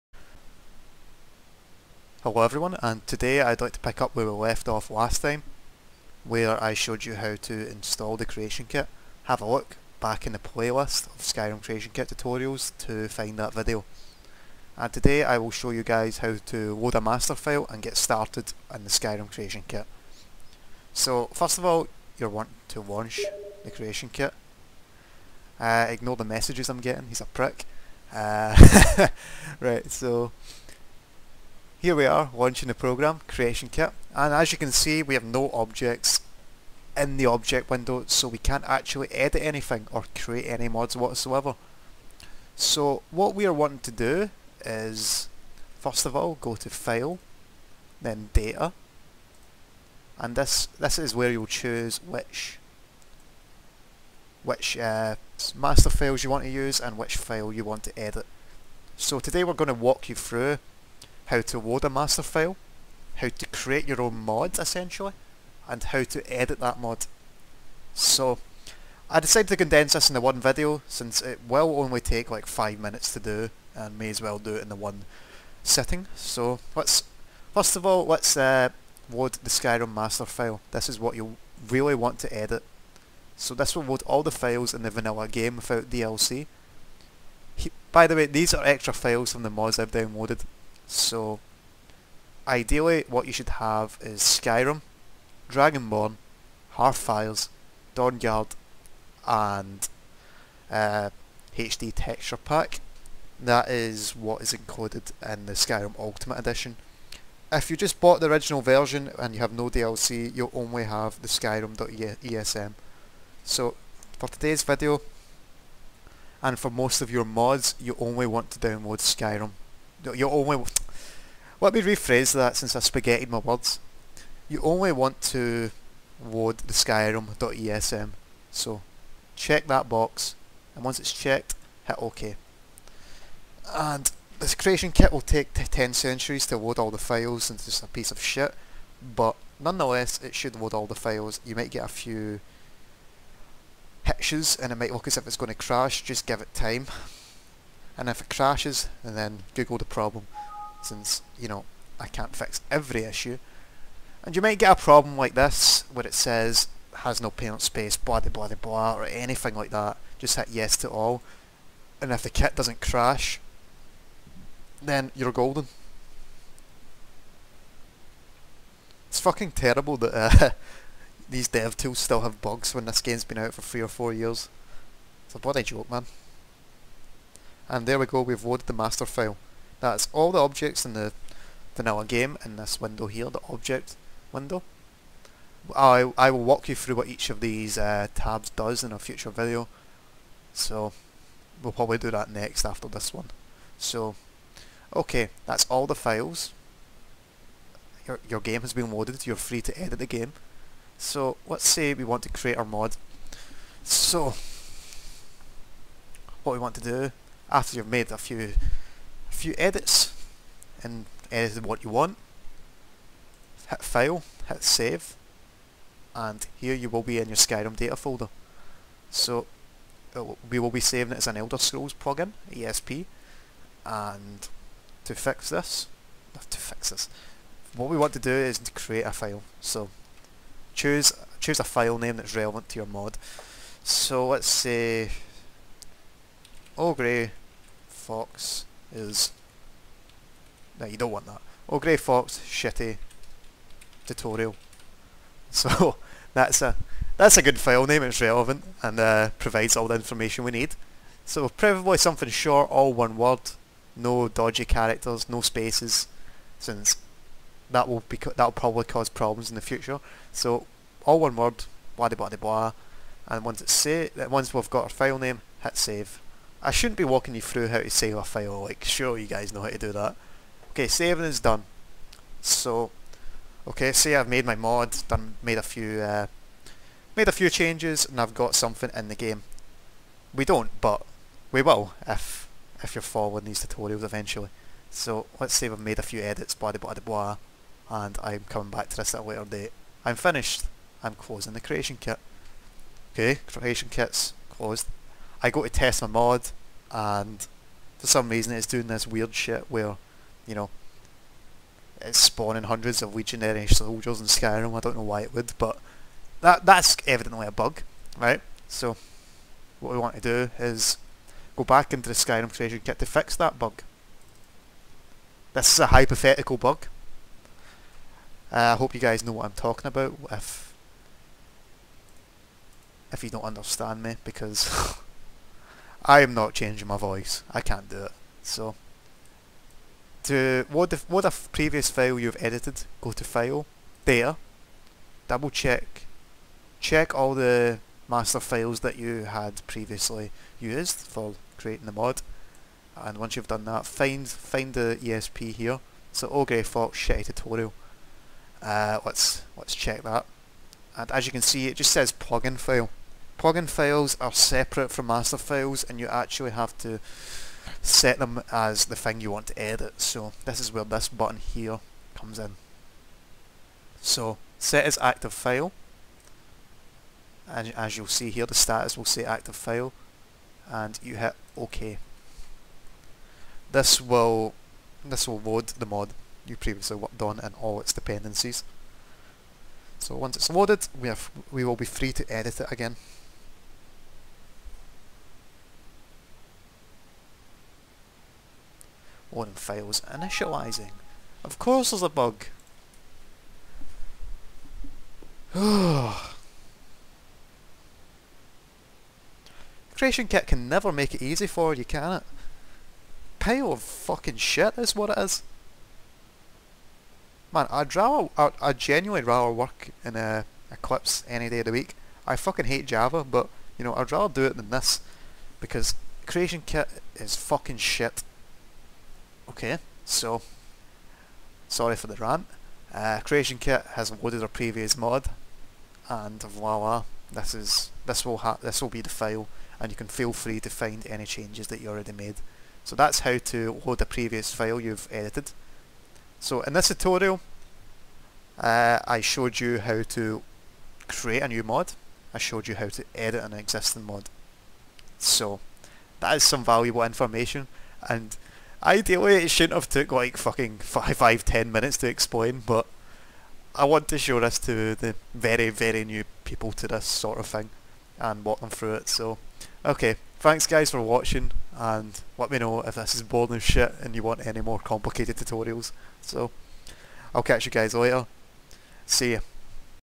Skyrim: Creation Kit Tutorials - Setting Up Your Project
I show you how to create a project and edit it later.